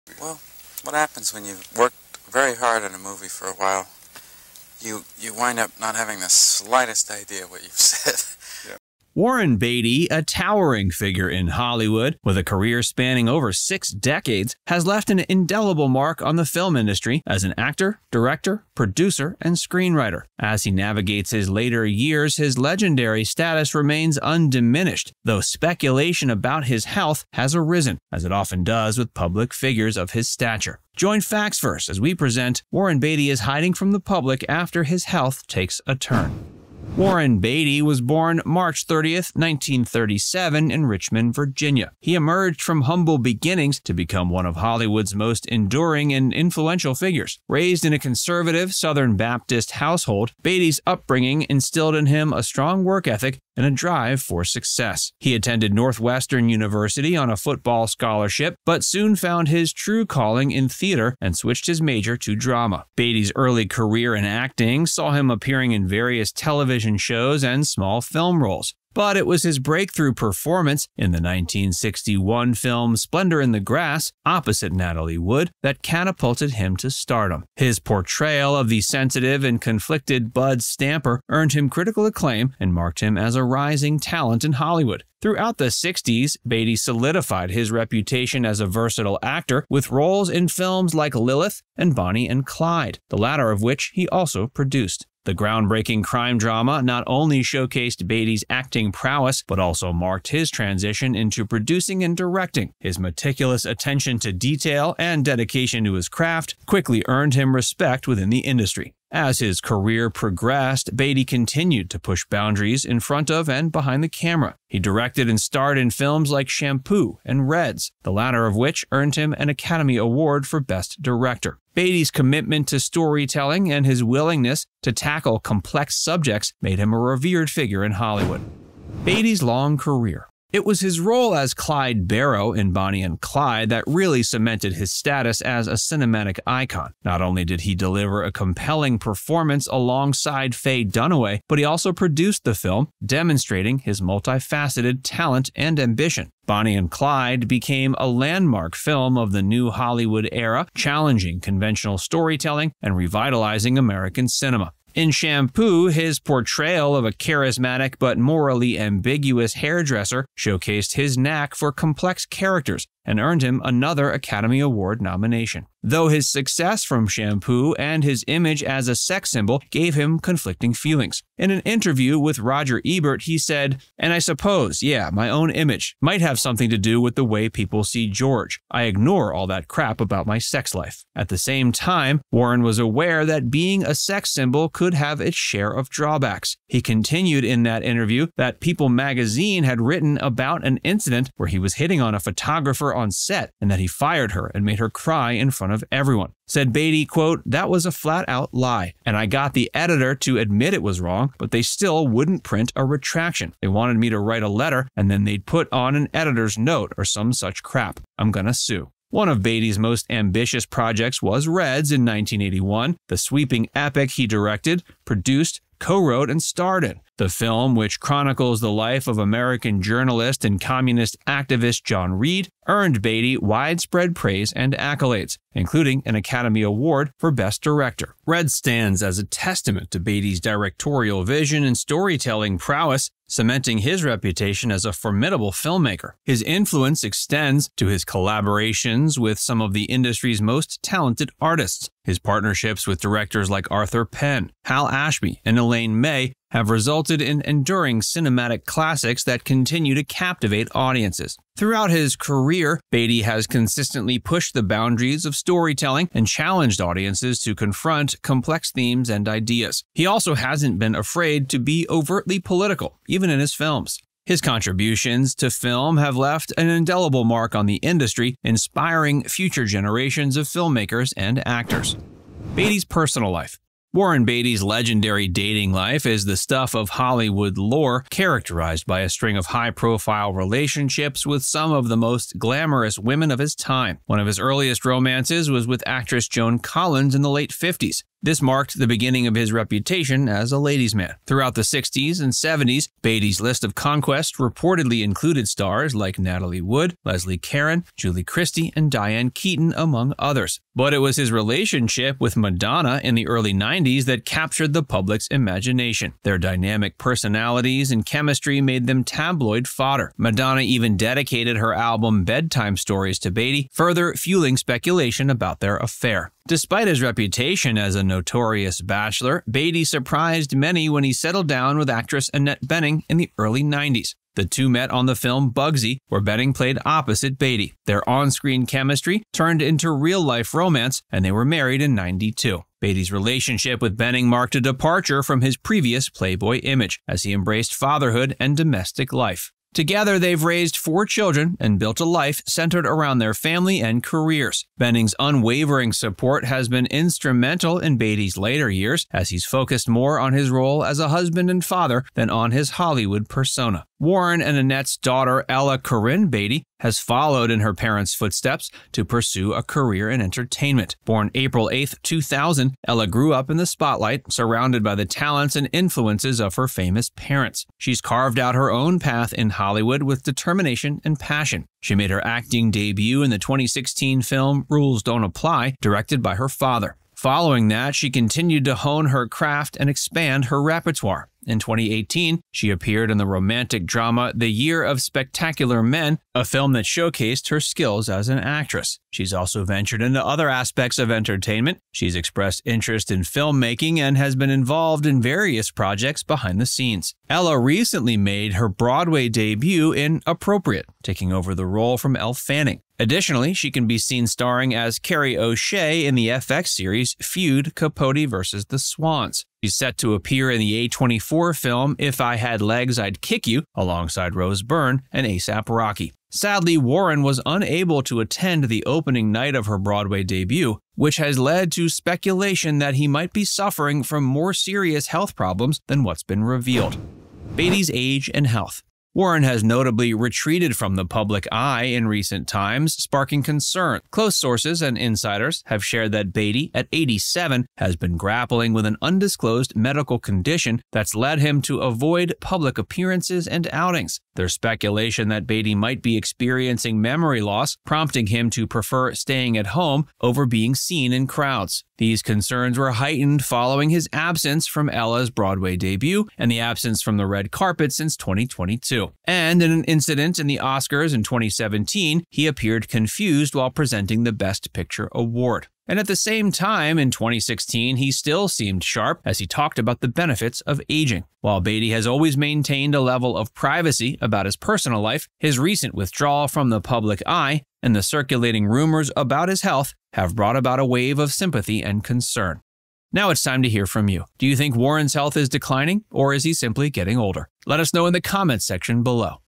Warren Beatty Is Hiding from the Public After His Health Takes a Turn
Warren Beatty, the iconic figure of American cinema, has carved out a multifaceted career that spans over six decades, leaving an indelible mark on the film industry. From his breakthrough role in "Splendor in the Grass" to his directorial triumphs like "Reds," Beatty has consistently showcased his talent as an actor, director, producer, and screenwriter.

00:00 - Intro
00:36 - Early Life
02:09 - Beatty’s Long Career
04:32 - Beatty’s Personal Life
07:07 - Beatty’s Health and Age
08:03 - Outro

His performances in groundbreaking films such as "Bonnie and Clyde" and "Shampoo" not only solidified his status as a leading man but also earned him critical acclaim and numerous accolades, including Academy Award nominations. However, as he gracefully enters his later years, whispers about his health have occasionally surfaced, as is often the case with revered public figures.

Speculation about Beatty's well-being has sparked concern among fans and industry insiders alike, prompting questions about his ability to continue contributing to the world of cinema. Despite these concerns, Beatty has remained relatively private about his health, leading to further speculation and conjecture. Yet, despite the passage of time and the inevitable toll it takes on the body, Beatty's enduring presence in Hollywood serves as a testament to his resilience and unwavering commitment to his craft.

His unparalleled contributions to film, coupled with his enigmatic persona, have solidified his status as a cinematic legend, revered by audiences and peers alike. As the speculation surrounding his health persists, Beatty's legacy remains untarnished, a testament to his enduring impact on the silver screen and his unwavering dedication to his art.

While the whispers of health concerns may cast a shadow over Beatty's illustrious career, they serve as a reminder of the human frailty that affects us all, regardless of fame or fortune. In the face of such speculation, Beatty's steadfast devotion to his craft and his refusal to be defined by rumors speak volumes about his character and integrity. As fans and admirers continue to celebrate his achievements and contributions to cinema, Warren Beatty's legacy will endure as a testament to the power of talent, perseverance, and passion.